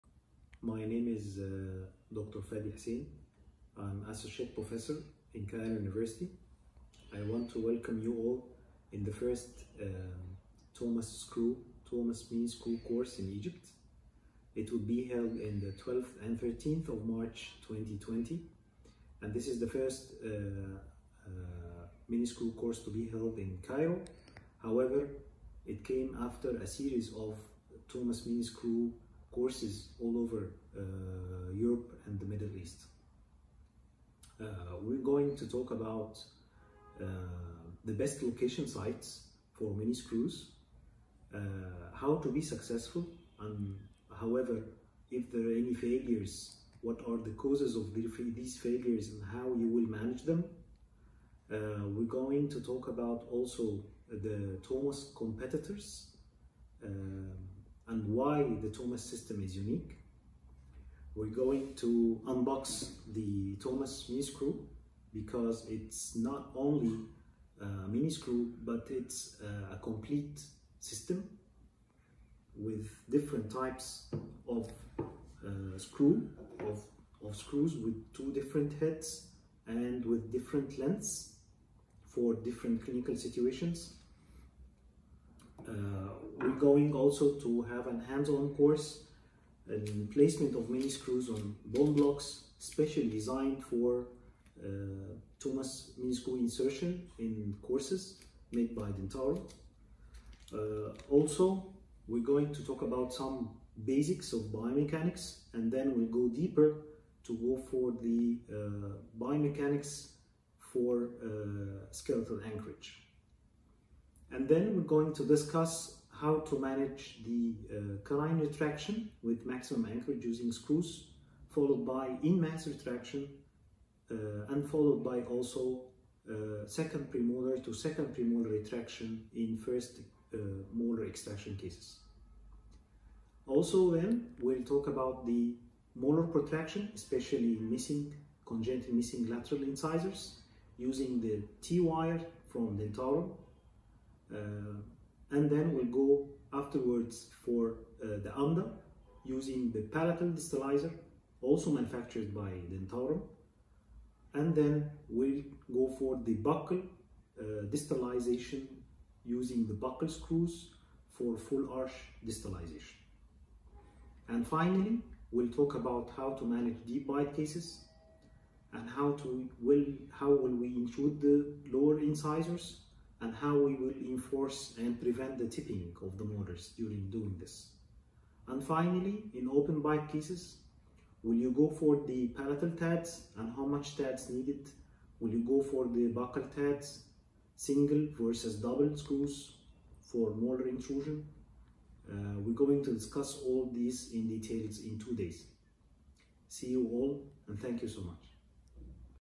First Tomas miniscrew course in Egypt Dentaurum Germany
Intro for the first tomas miniscrew course “Dentaurum  Germany” candidates will learn will learn all about Miniscrews for retraction distalization mesialization openbite and deepbite correction plus hands on